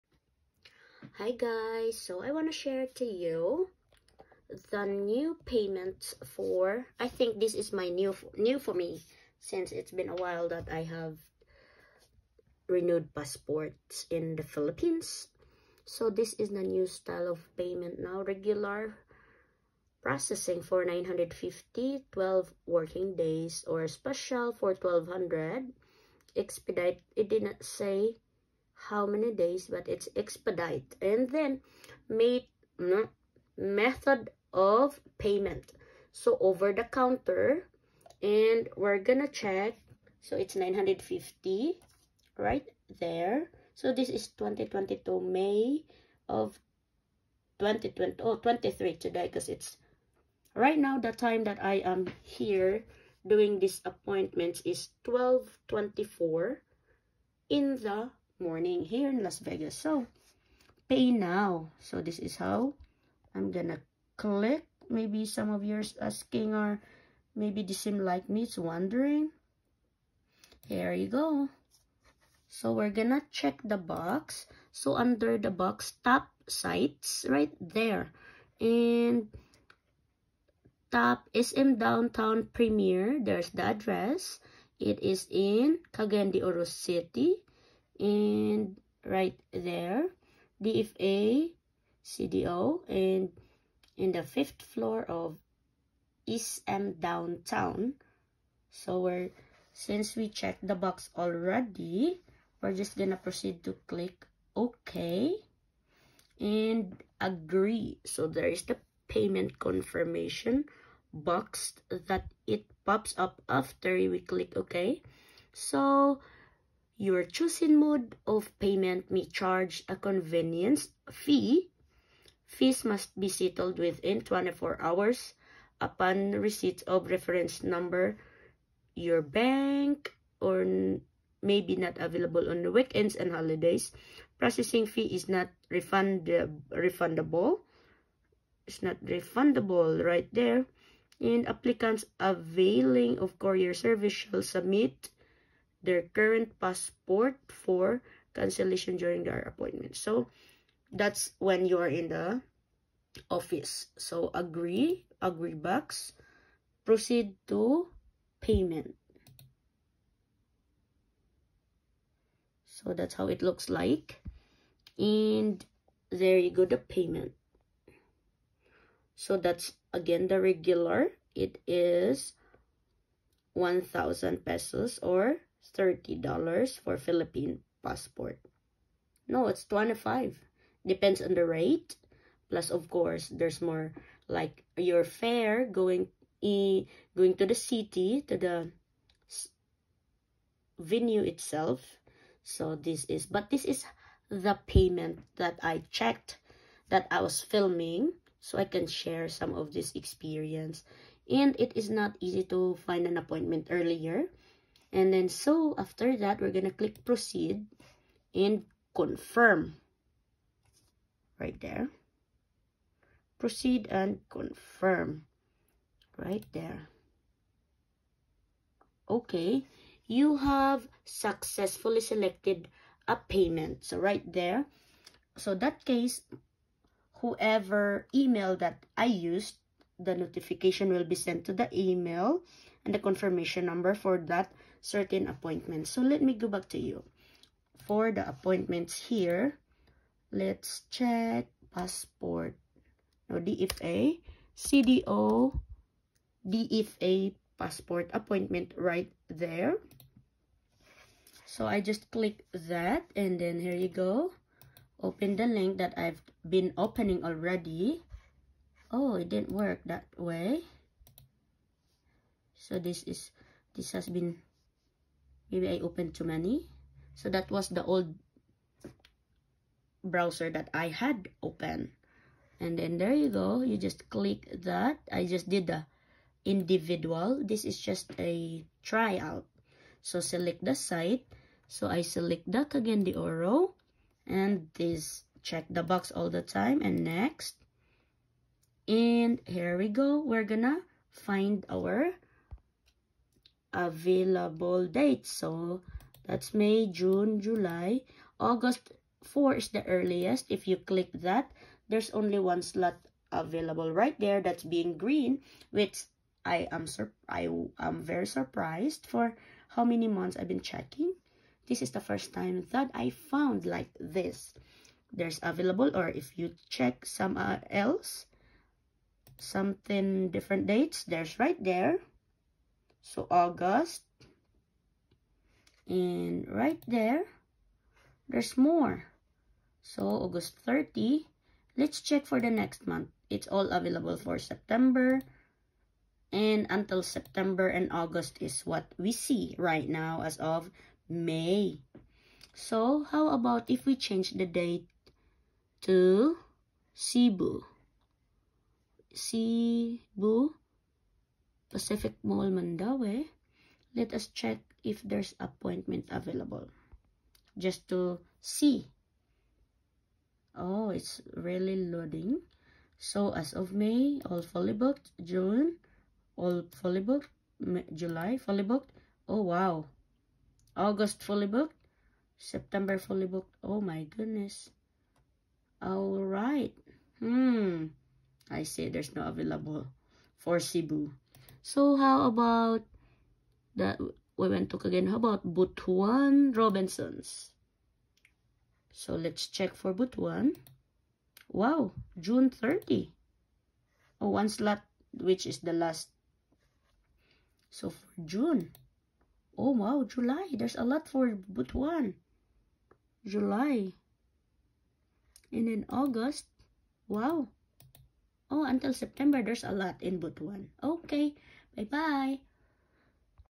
HOW MUCH IT COST FOR PASSPORT RENEWAL IN THE PHILIPPINES (HOW TO OBTAIN APPOINTMENT AND PAYMENT)
May 22,2022
I was on the site at 2:41 AM Las Vegas time(Pacific Time).
I may said some mistakes on my wordings on these video
(check on different sites, instead I said different dates).
It varies on your location for your available dates open or fully booked.
- I was doing a renewal of both adults so appointment was needed.
It was important that you have all the old passport information ready in front of you so your session won't get expired (30 minutes).
*last name
*given name
*date of birth... and more personal information of the applicant/s.
I had experienced with some items like province, occupation  but I just keep on retyping it until they accepted it, sometimes you need to keep trying until the site will respond and accept the inputs.

- minors and senior citizens are no need for appointment online.

MODE OF PAYMENTS WERE AVAILABLE OF YOUR CHOICE
and fees:
(1) regular processing cost 950 + 50 convenience = 1,000 pesos
(2) expedite for 1,200 pesos.

- just follow the links sent to your given email to them for the payments and confirmation, you have 24hrs to pay your chosen slot.

-UNEDITED CLIPS
-UNPLANNED